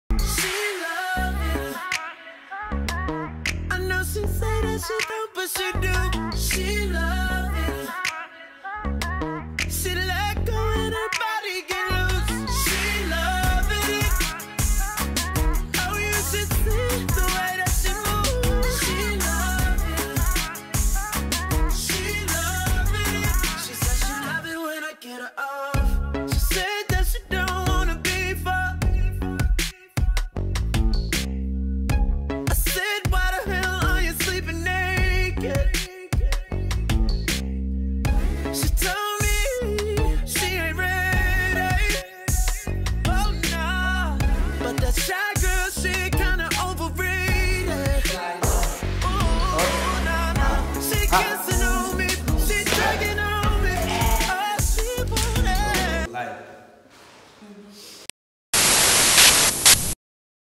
Behind the scenes w/ Vybez Dancers (Unreleased footage)
Check out some behind the scenes of Choreographer Keith Andre working with the @Vybezdancers on there latest performance @ Vybez showcase ATL.  Follow the ladies @VYBEZDANCERS